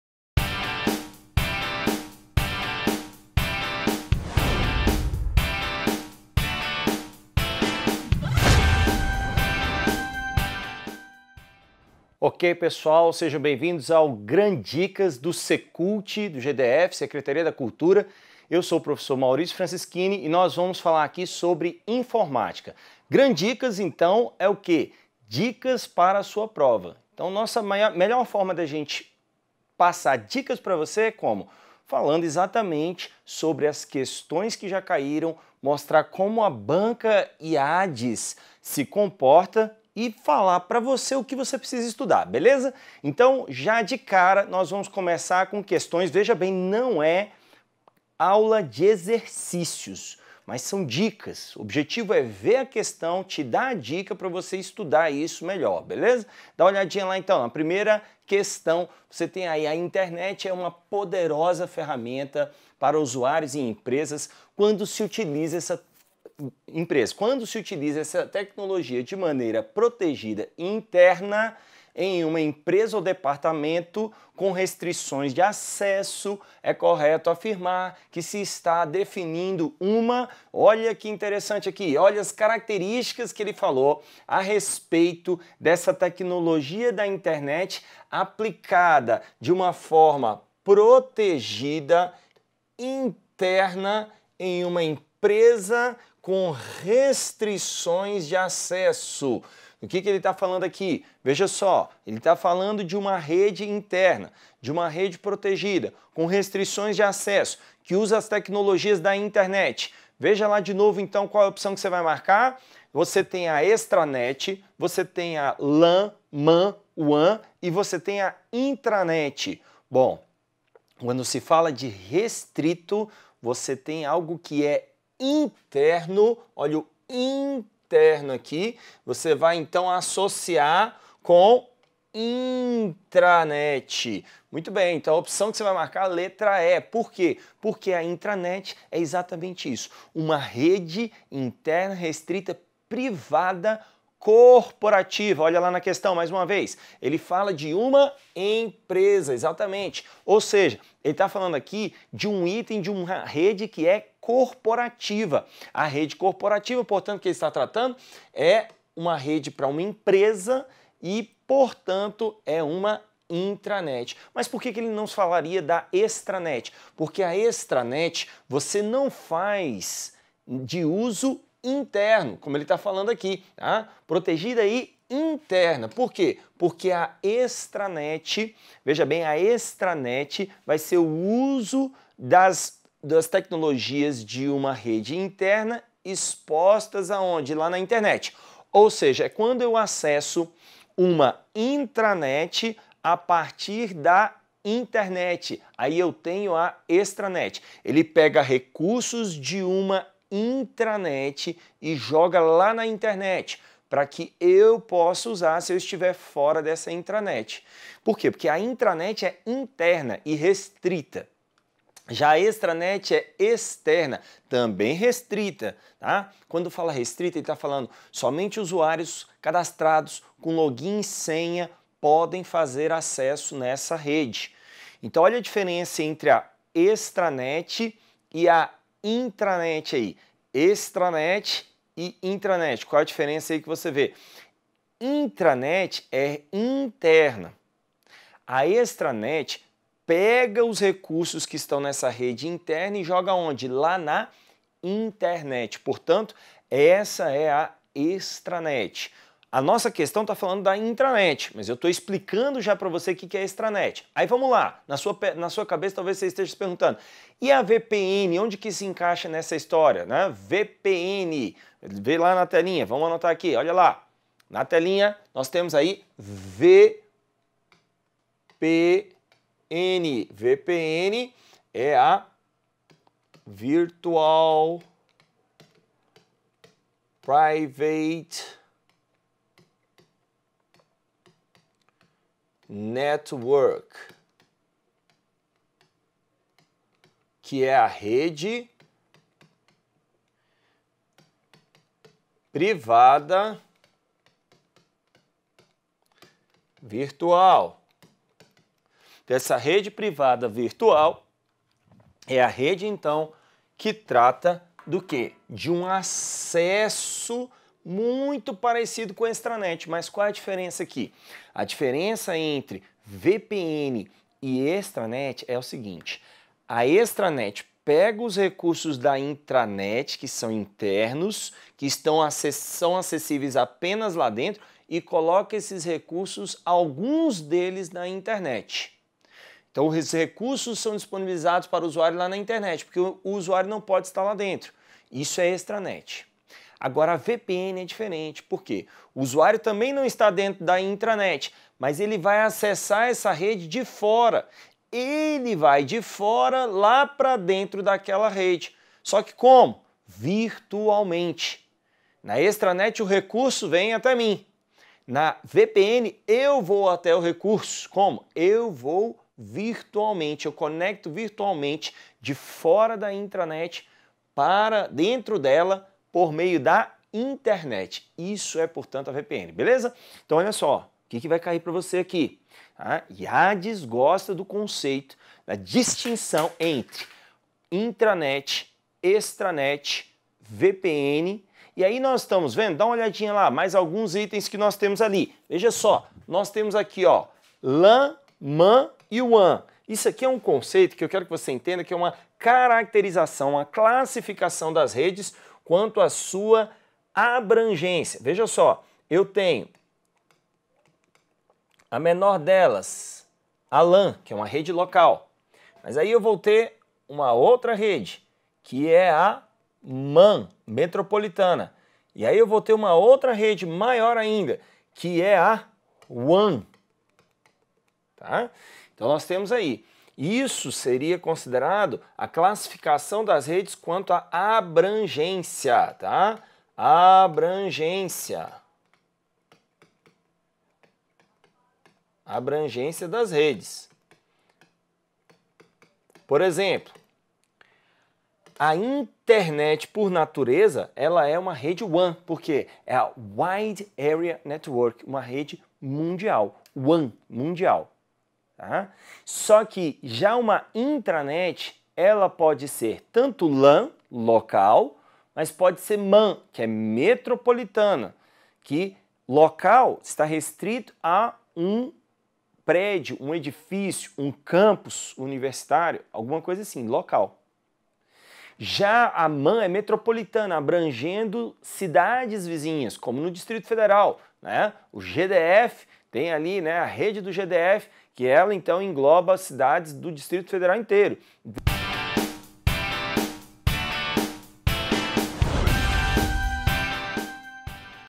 Informática Gran Dicas Secult - Maurício Franceschini - Aula Gratuita
Vídeo Demonstrativo de Informática Gran Dicas para Secult com o professor Maurício Franceschini

Conheça o Gran Cursos e conquiste o cargo de seus sonhos. Confira os nossos cursos preparatórios e materiais didáticos.

Gran Cursos - 24 anos de tradição.